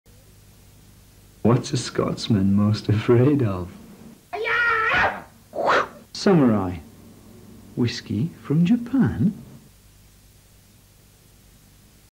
Samurai whisky from Japan 1985
A commercial from the Creative Circle 'Role Reversal Seminar' Trinity collage, Oxford. 1985